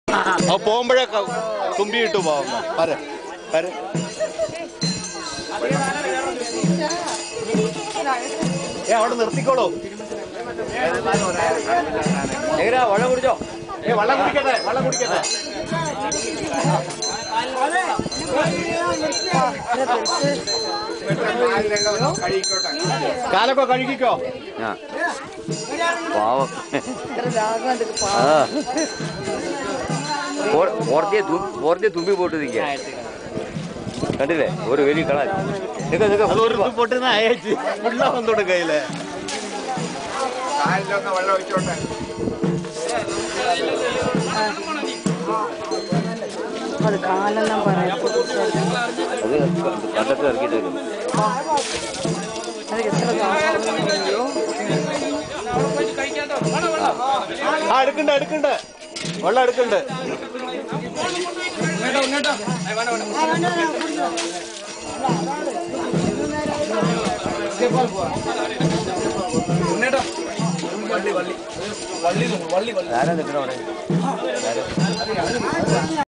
Pallavur Ezham Vilakku Festival 2010 - Elephant climbing steps
Elephant climbing steps at Pallavur Navarathri Ezham Vilakku Festival, October 2010